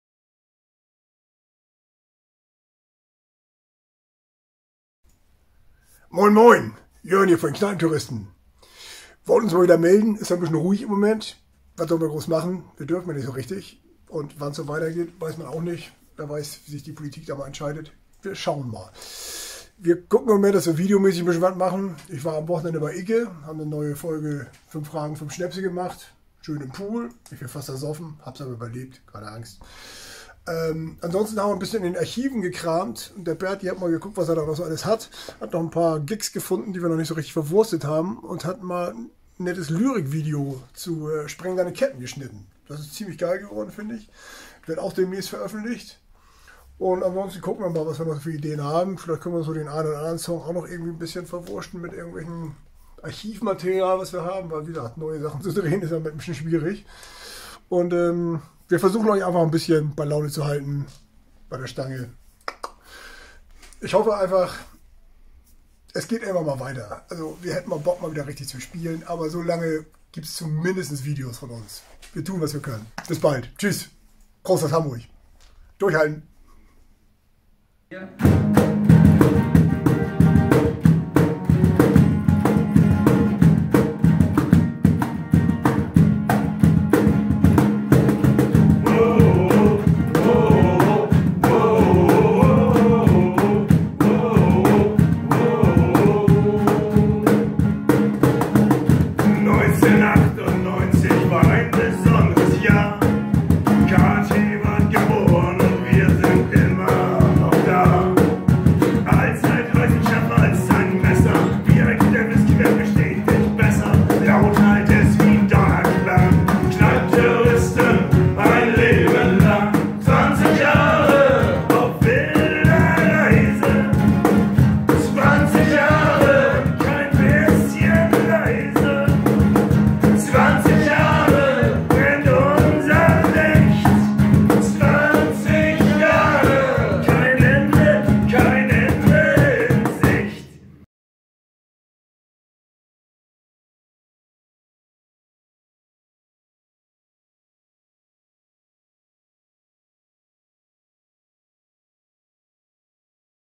Neues von den KneipenTerroristen
Es gibt wieder NEUES von uns KneipenTerroristen.
Unser Sänger Jörn, hat das für EUCH in einem kurzen Video zusammengefasst und am Ende gibt es noch ein wenig Musik.
Viel Spaß beim Video und ein dickes PROST aus Hamburg.

Falls Ihr noch mehr geiles Kneipenterroristen Merch haben wollt.